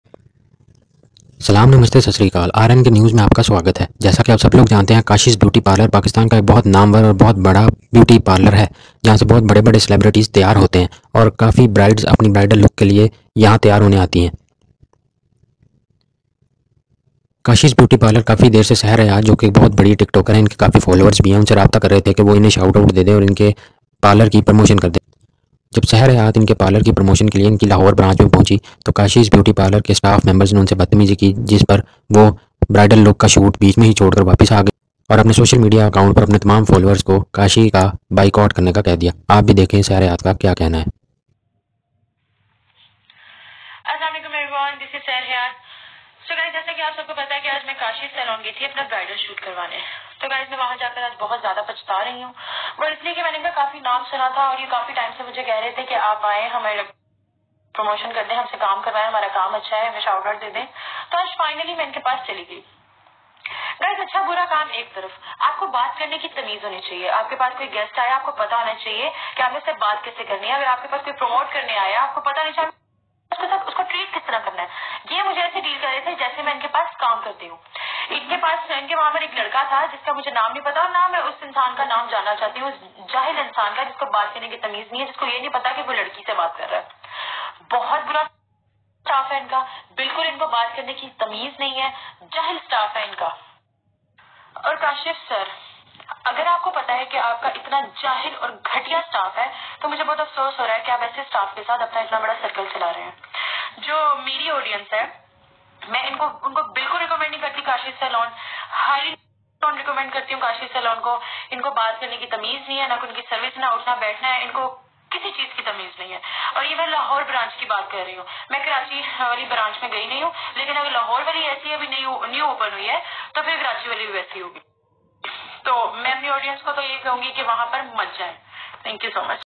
kashees beauty parlour ( Lahore branch) staff members misbehaves with sehar hayat.2022.
kashees beauty parlour staff misbehaves with famous tiktoker of Pakistan sehar hayat. firstly the invited her for promotion then insulted her and misbehaves with her.Sehar hayat in his Instagram story told all the story about what happens there. watch video to know more.